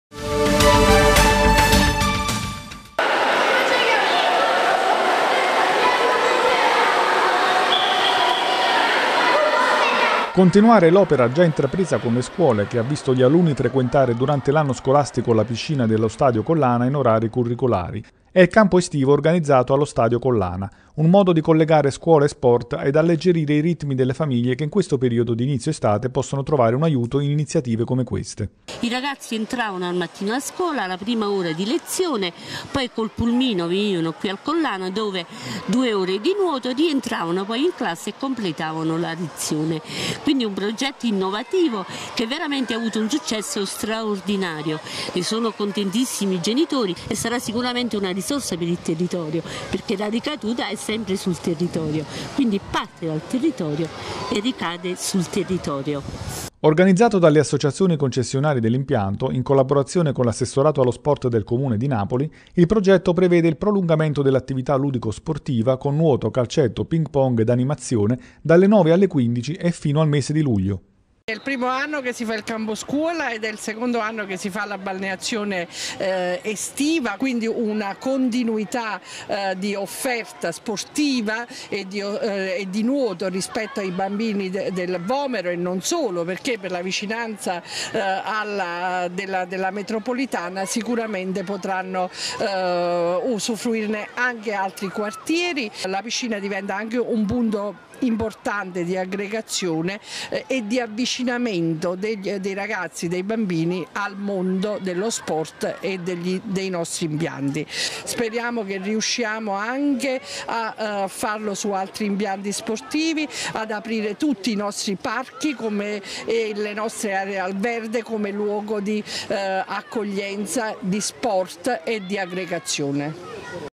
Al Collana si apre il campo estivo
Dopo l'apertura della piscina in orari curriculari, ecco il campo estivo organizzato allo Stadio Collana, per collegare scuola ed sport ed alleggerire i ritmi delle famiglie: nuoto, calcetto, ping pong ed animazione, dalle 9 alle 15 e fino al mese di luglio.